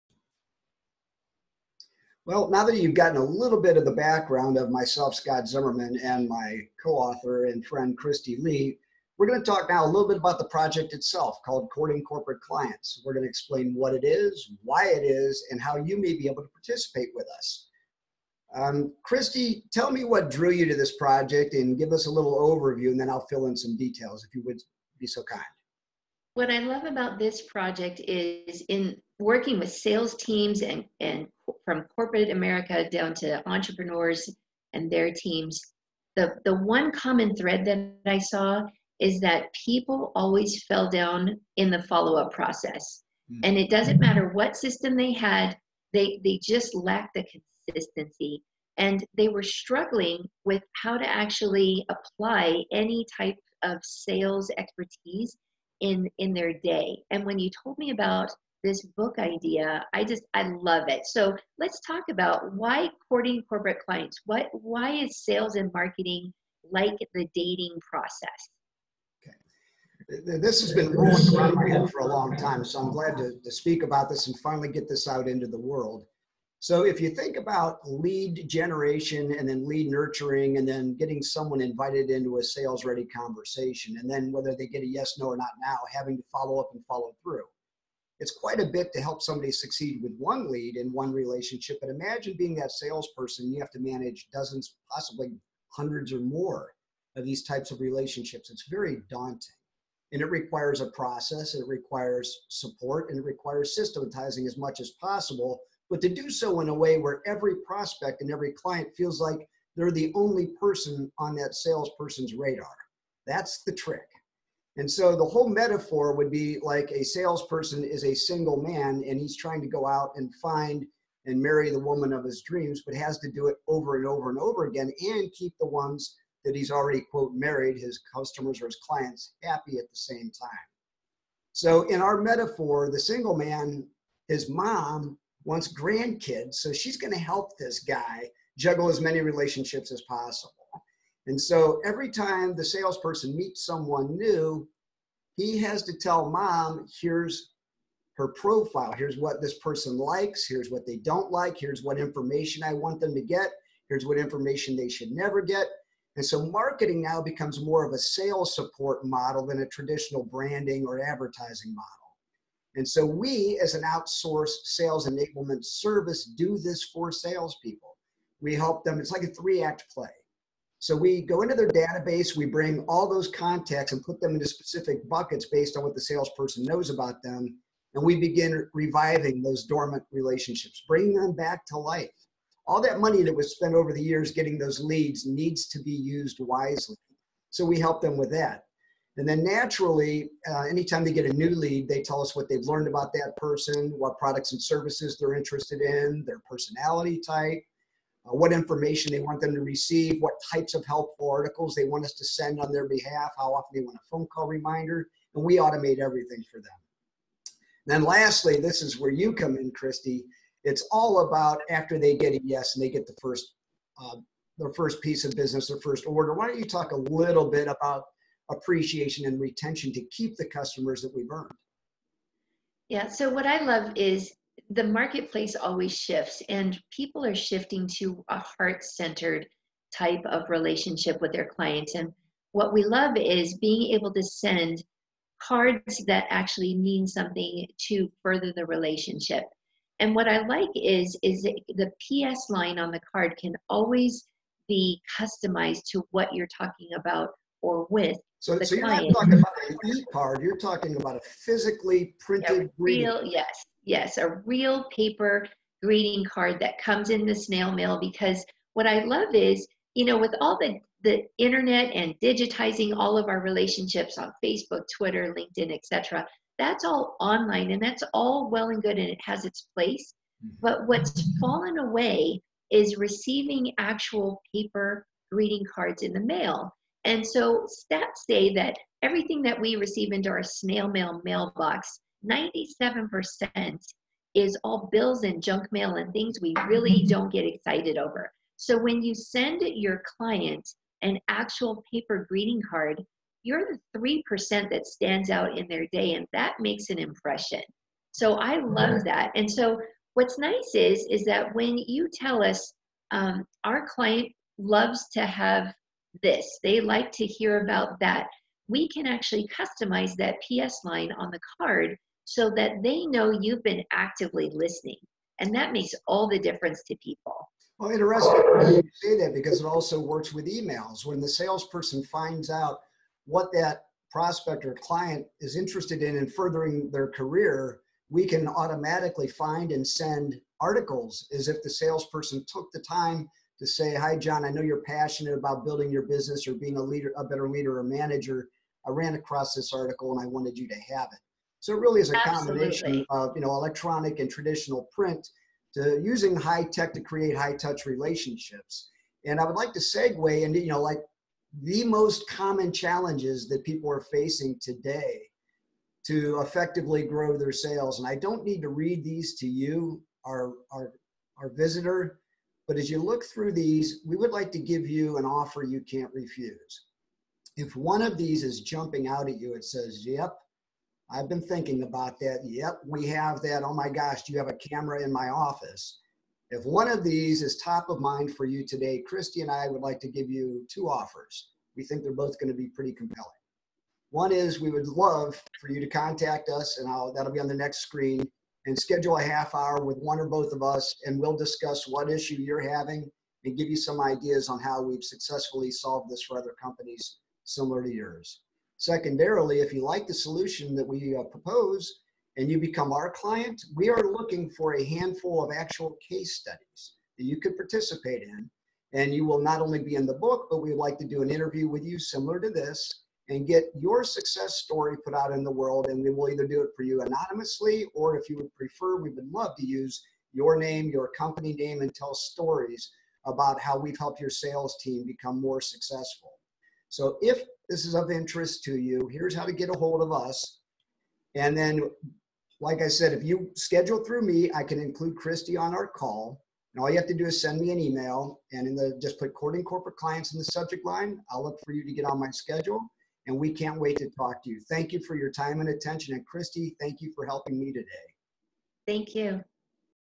2 of 5: Courting Corporate Clients - Overview
Scott Zimmerman and Kristi Lee discuss the metaphor for their book, Courting Corporate Clients, how selling is like dating. It's hard enough to find, court, and marry one person - imagine a hundred people!